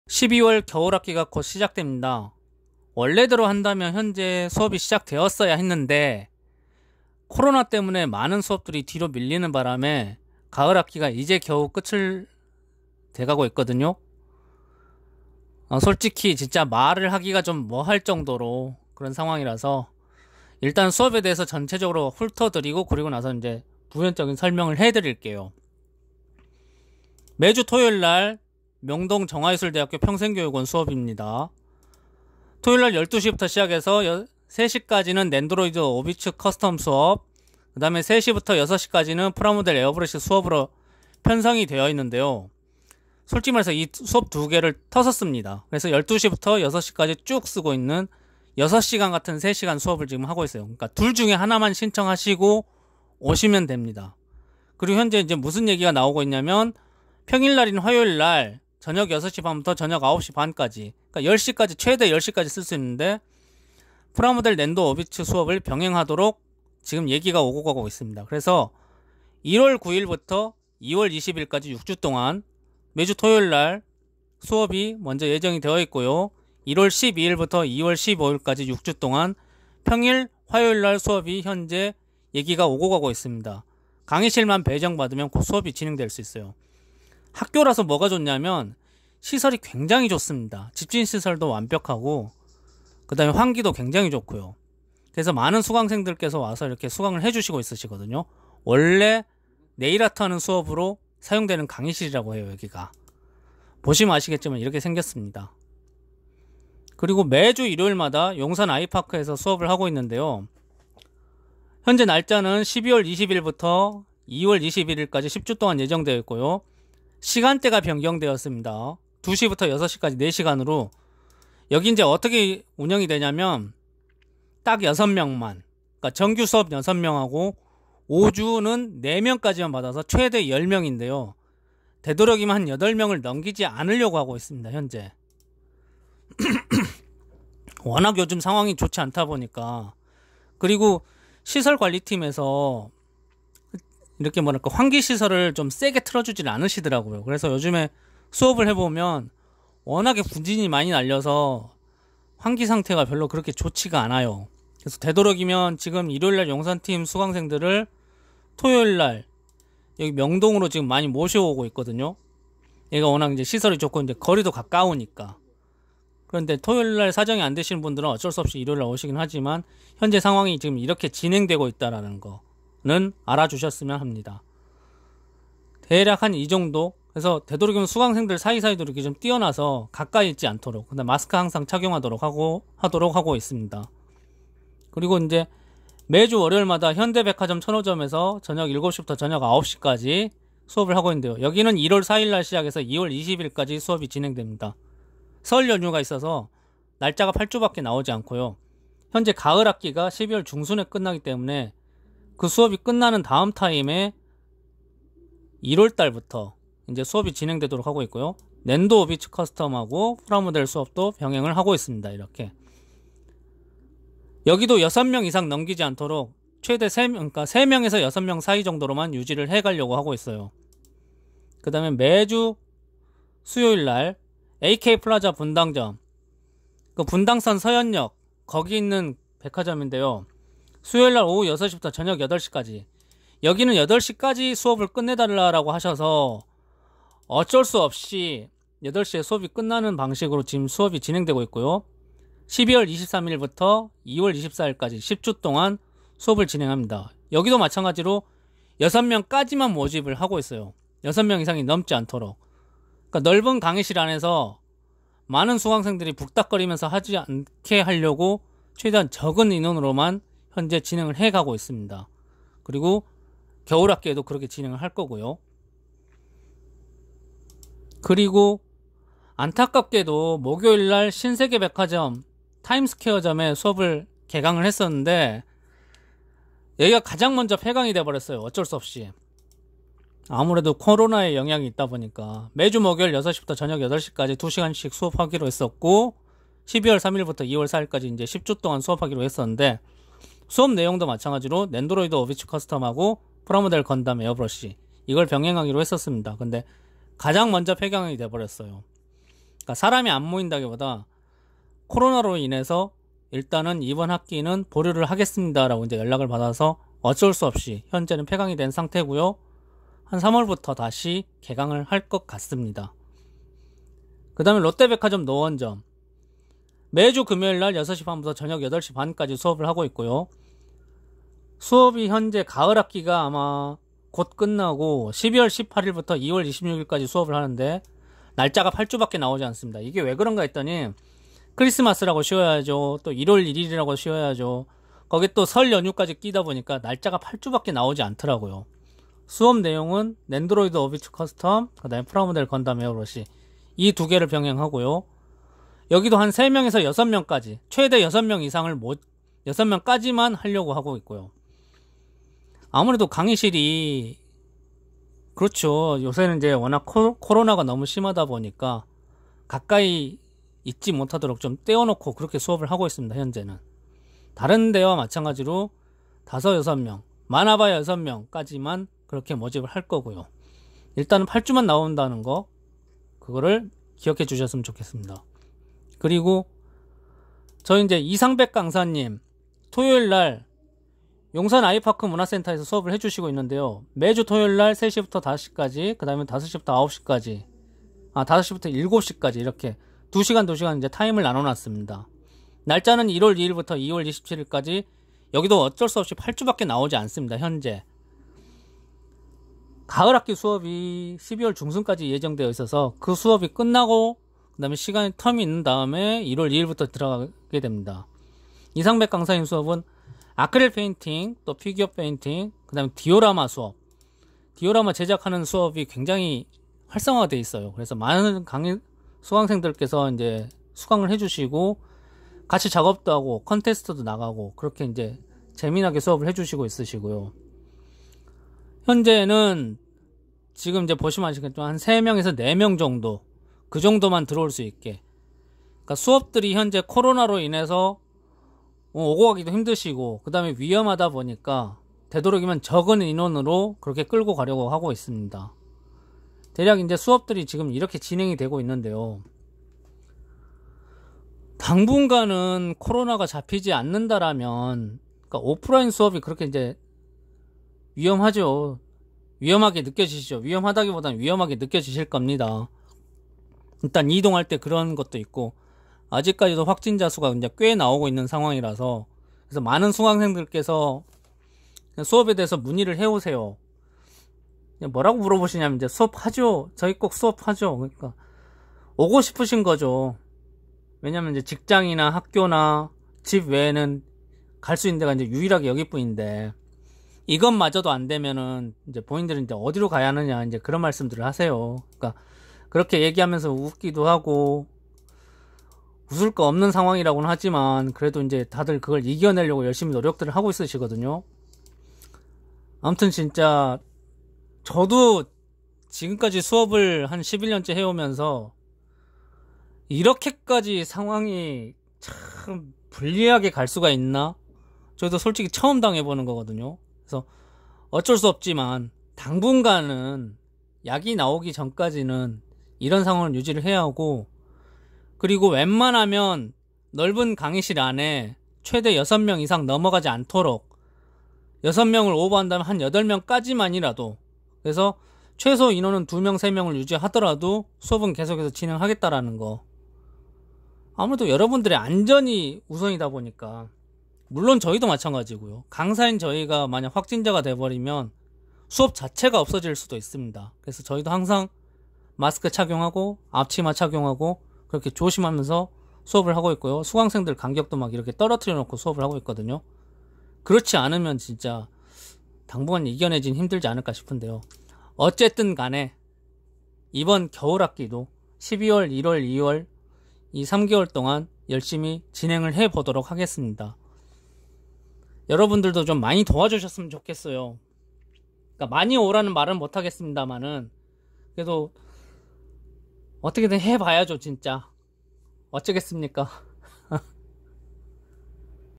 튜닝타임즈 프라모델 강좌 2020/2021년 12/1월 겨울학기 넨도로이드 오비츠 커스텀 & 프라모델 에어브러시 수업 안내
명동 정화예술대학교 평생교육원
매주 토요일 12:00 ~ 15:00 (3시간) 넨도로이드 오비츠 커스텀
매주 토요일 15:00 ~ 18:00 (3시간) 프라모델 에어브러시
매주 화요일 18:30 ~ 21:30 (3시간) 프라모델 넨도 오비츠 병행

2021년 1월 9일 ~ 2월 20일 (6주)
2021년 1월 12일 ~ 2월 15일 (6주)

용산 아이파크문화센터
매주 일요일 14:00 ~ 18:00 (4시간)
2020년 12월 20일 ~ 2021년 2월 21일 (10주)

넨도로이드 오비츠 커스텀, 프라모델 건담 에어브러시 병행

검색에 강사님 이름 김두영 입력/ 인터넷 안뜨면 당황하지 말고 전화로 문의

현대백화점문화센터 천호점
매주 월요일 19:00 ~ 21:00 (2시간)
2021년 1월 4일 ~ 2월 22일 (8주)

AK플라자문화센터 분당점
매주 수요일 18:00 ~ 20:00 (2시간)
2020년 12월 23일 ~ 2021년 2월 24일 (10주)

신세계백화점문화센터 타임스퀘어점
매주 목요일 18:00 ~ 20:00 (2시간)
2020년 12월 3일 ~ 2021년 2월 4일 (10주)

롯데백화점문화센터 노원점
매주 금요일 18:30 ~ 20:30 (2시간)
2020년 12월 18일 ~ 2021년 2월 26일 (8주)

전화문의 및 수강신청: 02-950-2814~5

이상백 강사님의 용산 아이파크 문화센터
매주 토요일 15:00 ~ 17:00 (2시간)
매주 토요일 17:00 ~ 19:00 (2시간)
2021년 1월 2일 ~ 2월 27일 (8주)

아크릴 페인팅, 피규어 페인팅, 건담 디오라마, 웨더링 수업

검색에 강사님 이름 이상백 입력/ 인터넷 안뜨면 당황하지 말고 전화로 문의